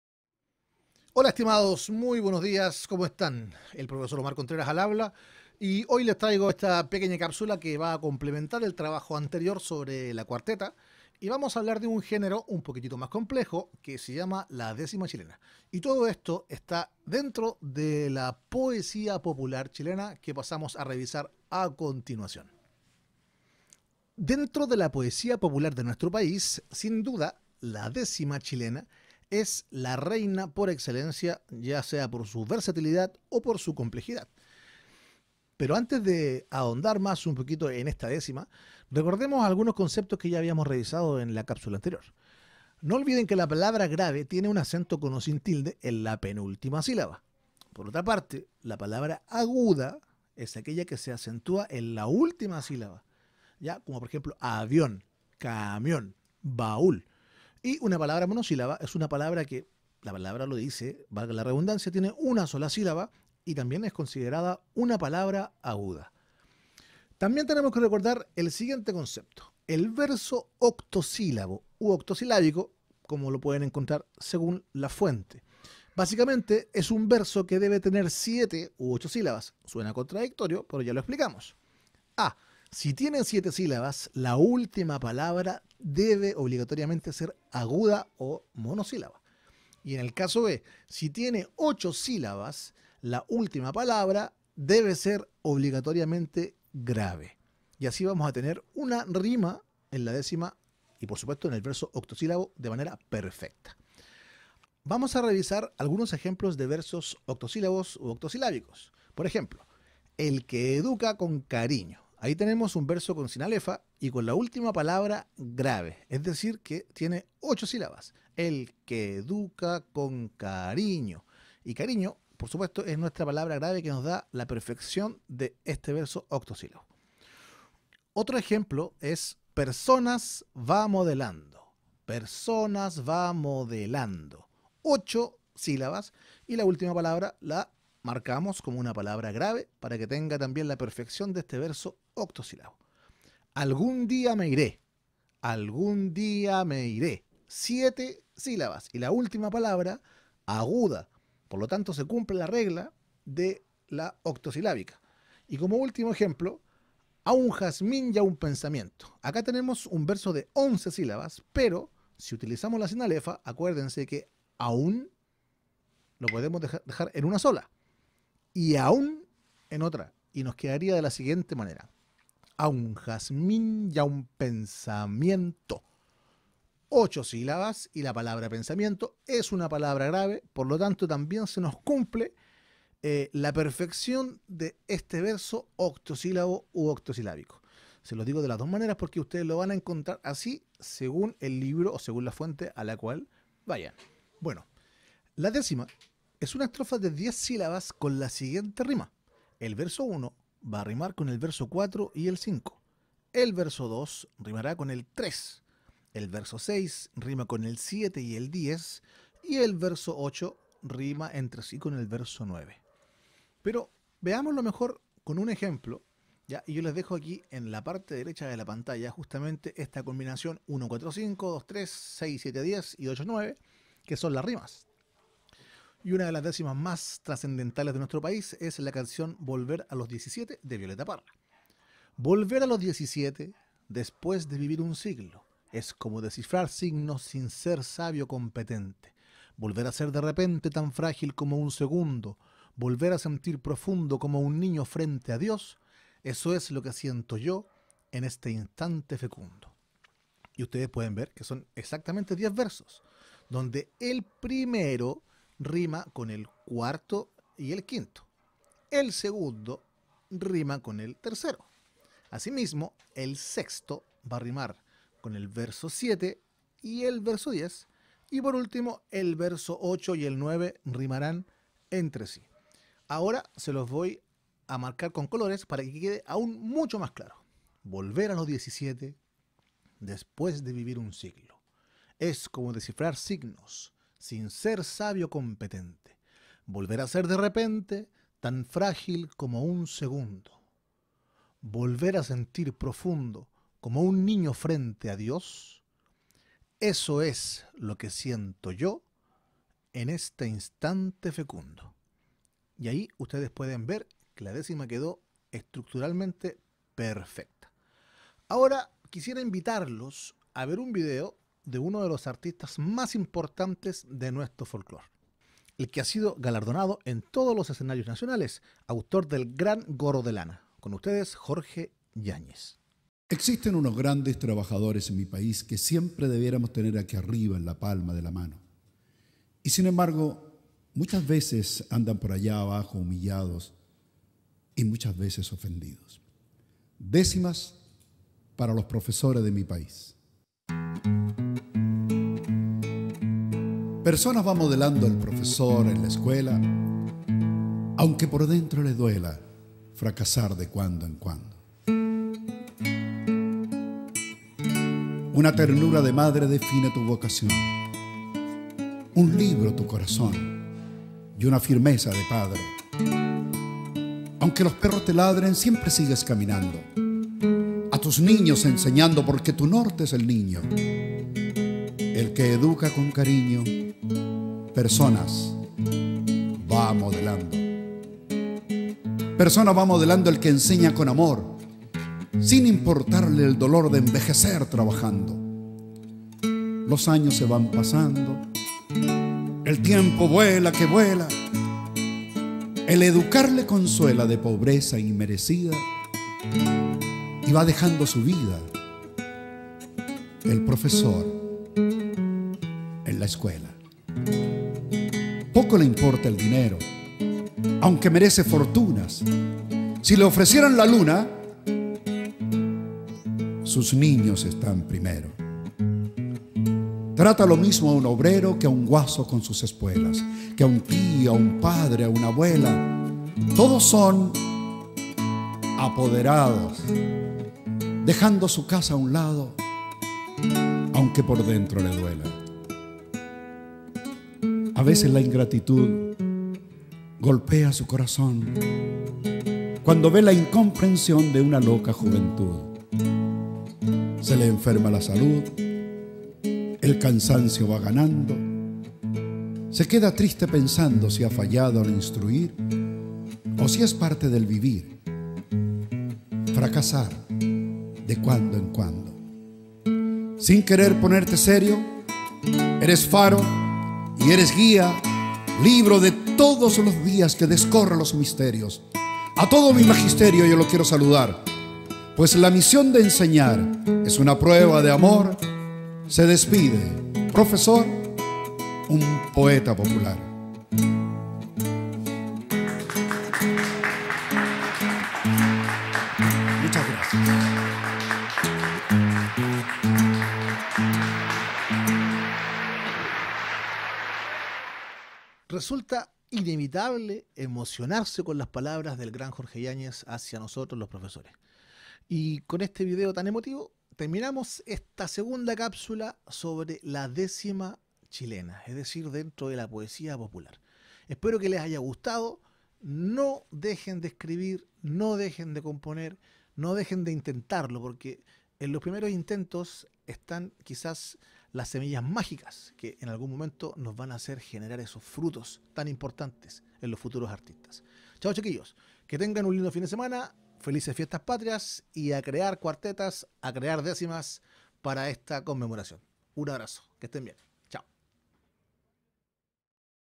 La décima
Para aprender a crear décimas de la poesía popular Chilena